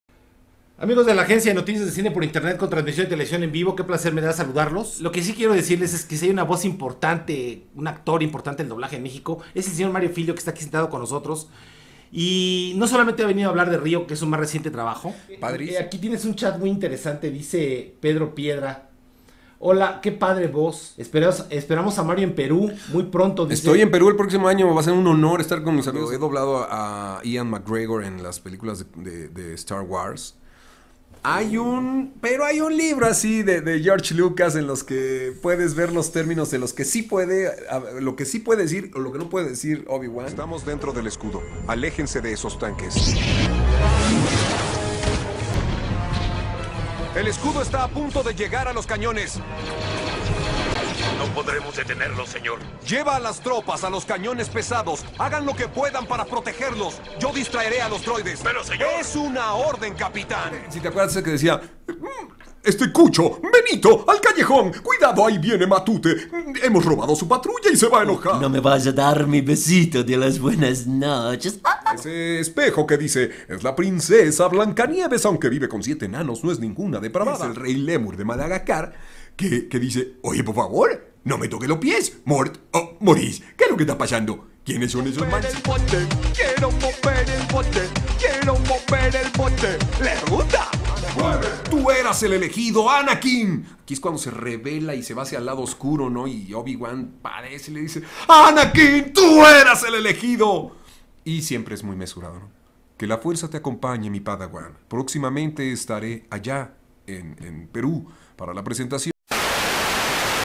Entrevista: Mario Filio (la voz de Obi Wan en castellano) viene a Perú ?
Según este video, una entrevista al actor de doblaje Mario Filio, éste hace referencia a una visita a Perú el año próximo. Es decir, el año 2012.

Mario Filio es un reconocido actor de doblaje mexicano. Es más conocido por haber sido la voz de Obi-Wan Kenobi en Star Wars Episodios II y III, Clone Wars (Micro-serie), The Clone Wars (la película), y The Clone Wars (serie animada); así como el Rey Julien XIII de la película y cortos animados como Madagascar y Jaimico en la serie animada Soy la Comadreja.